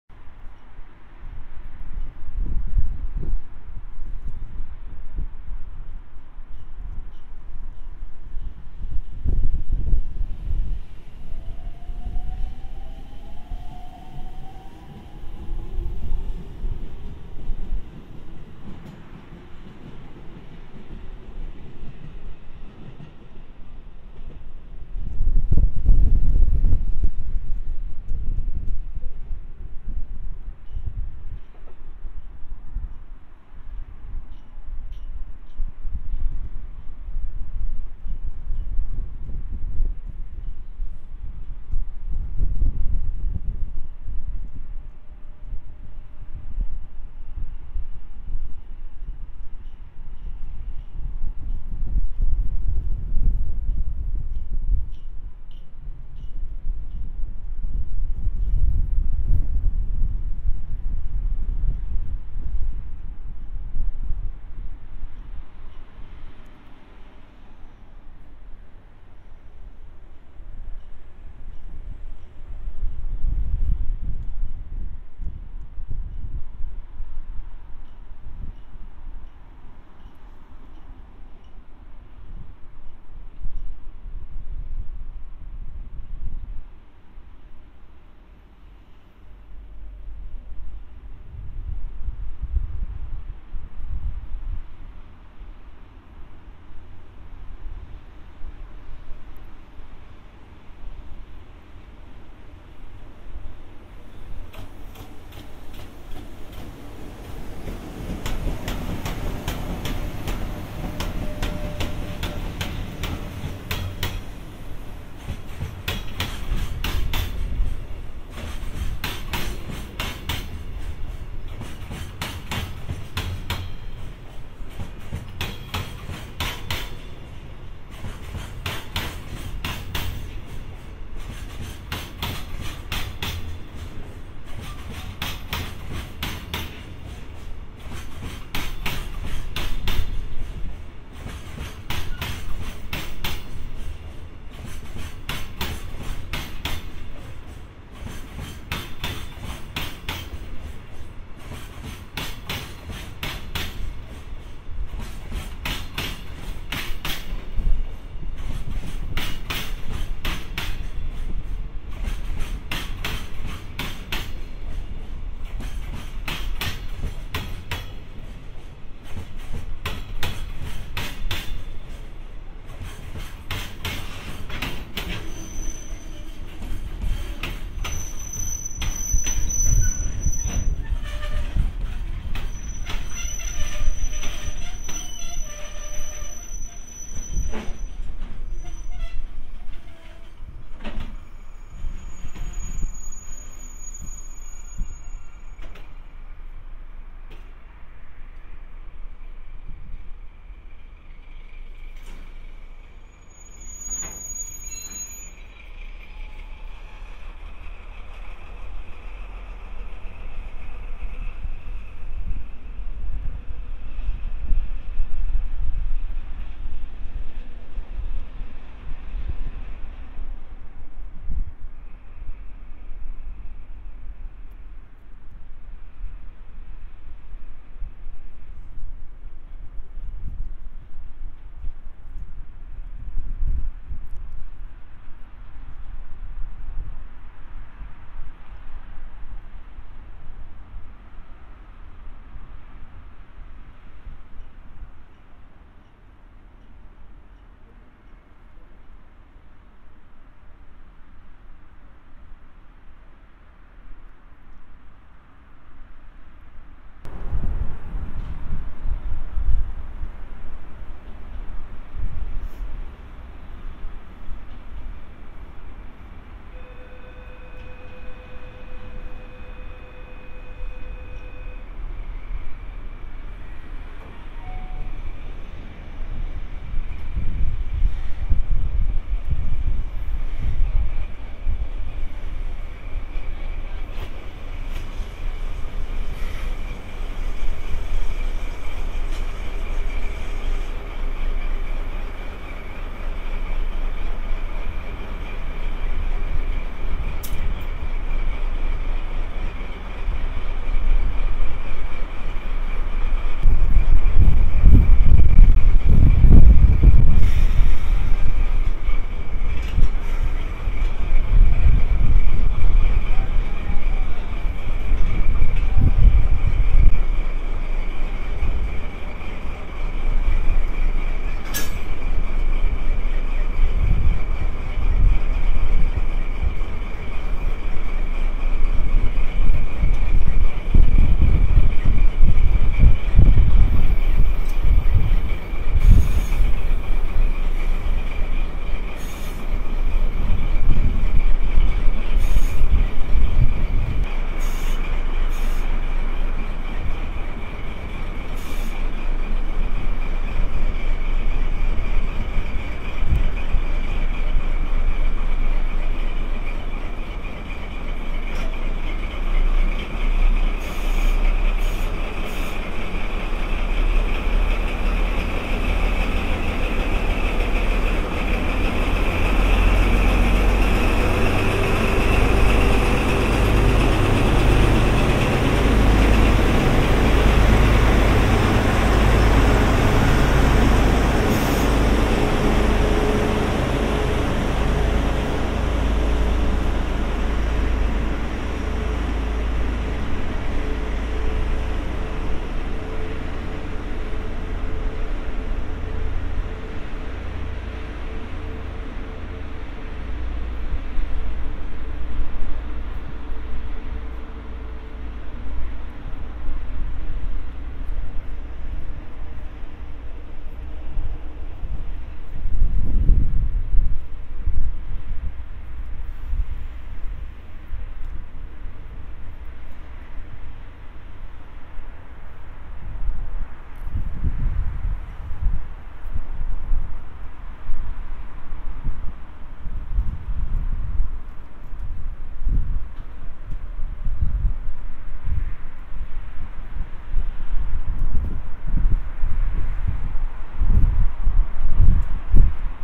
2015/01/17 【ゼロロク重連】 JR貨物 3091レ EF66 24 + EF66 36 名古屋貨物ターミナル駅 | JR Freight: Cargo by EF66 at Nagoya
JR貨物
西名古屋港線
3091レ コンテナ
仙台タ ～ 名古屋タ
EF66 24 + EF66 36
名古屋貨物ターミナル駅
2015年1月17日